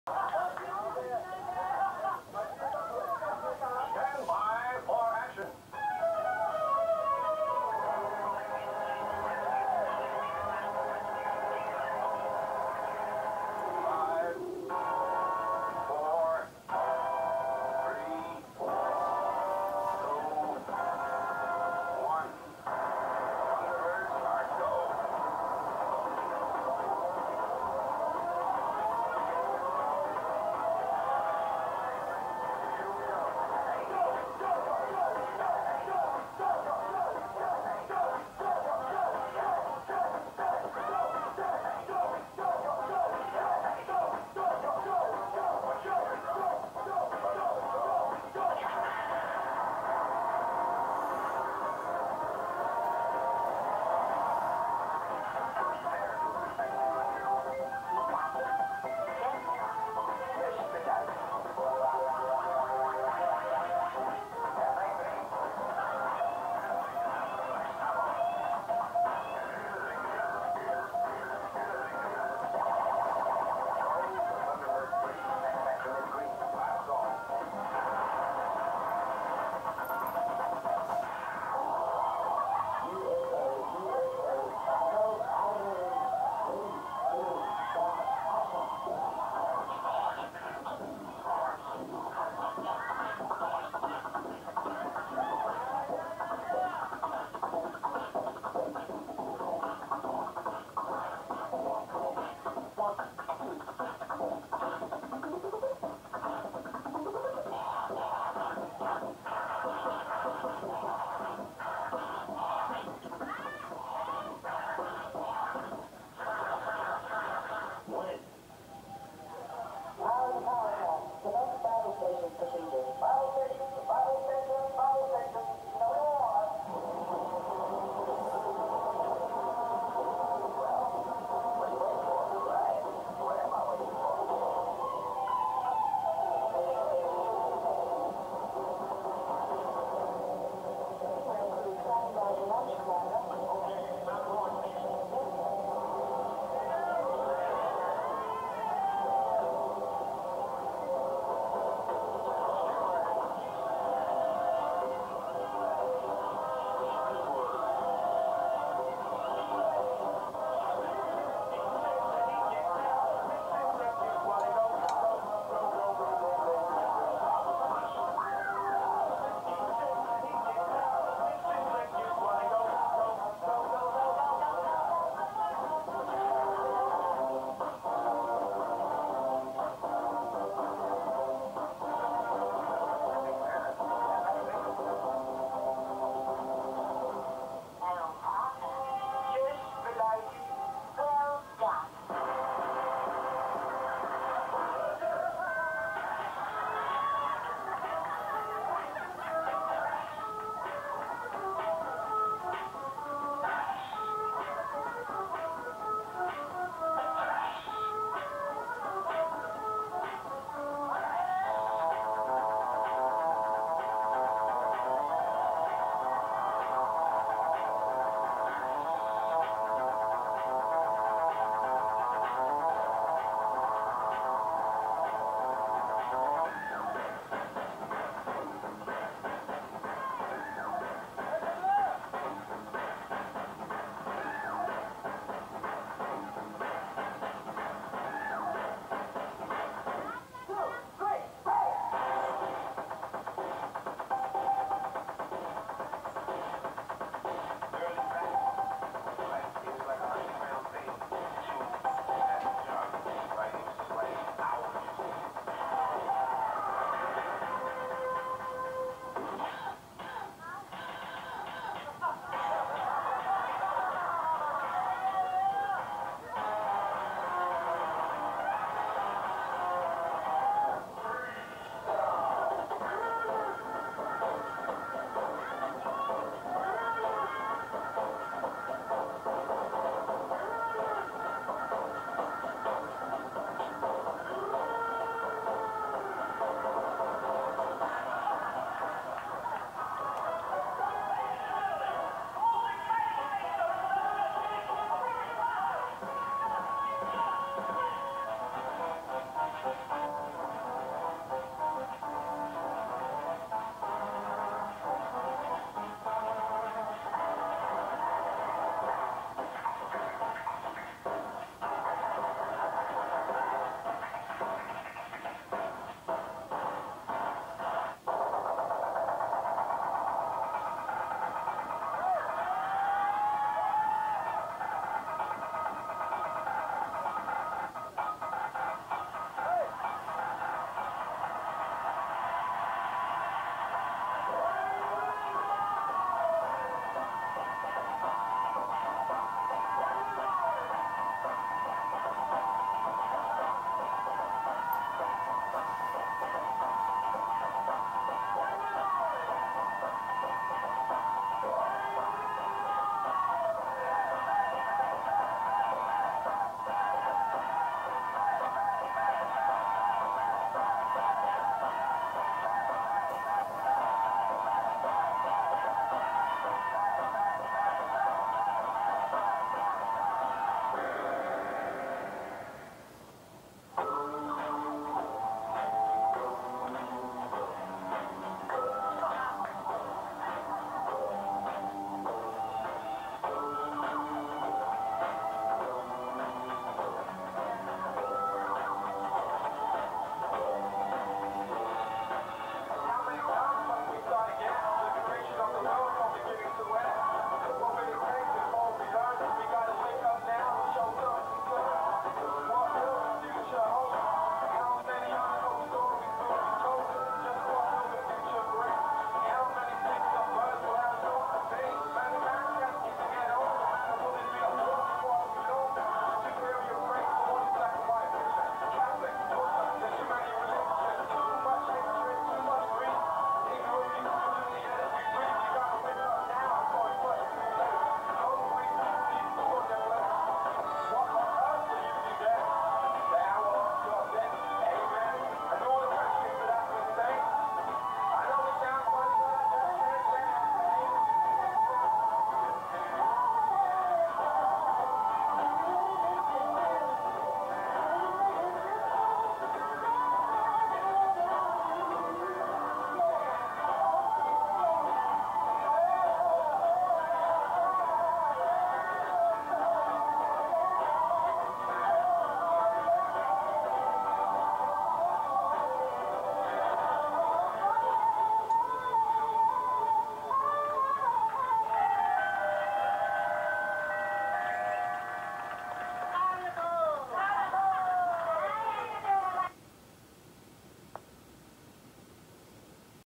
【SPECIAL-GUEST】サンダーバードネタ
スペゲスのサンダーバードネタ。

ダンス番組「RAVE2001」ではショートバージョンでしたが、実はもっと長いネタでしたw

【SPECIAL-GUEST】
サンダーバードネタ

⚫︎東京ダンスシーン
@六本木 イベント

（※VHS時代の映像の為、画質＆音質:低🙏）

では！!いきます˚✧₊⁎❝᷀ົཽ≀ˍ̮ ❝᷀ົཽ⁎⁺˳✧༚